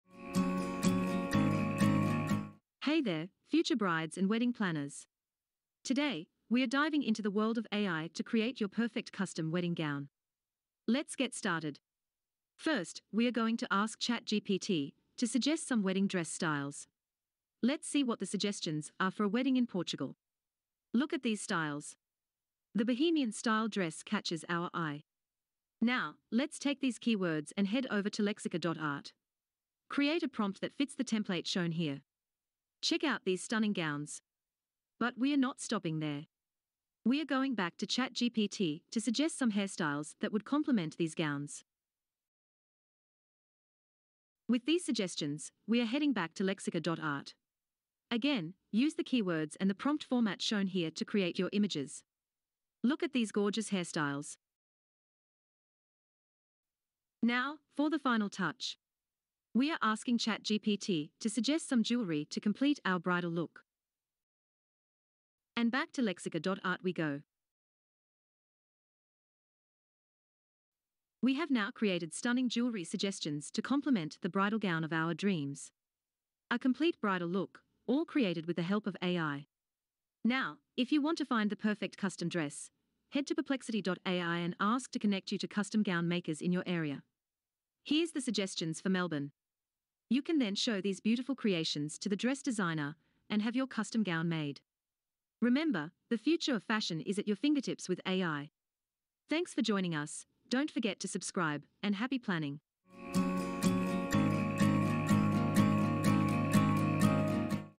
We Used AI to Design a Wedding Dress! Here's What Happened...
Join us as we explore the future of bridal fashion with AI! In this video, we use ChatGPT and Lexica.art, two powerful AI tools, to create a complete bridal look. We start by generating unique wedding gown styles, then move on to matching hairstyles, and finally, the perfect jewelry to complete the look. Whether you're a bride-to-be or a fashion enthusiast, you'll love seeing how AI can transform the way we envision style.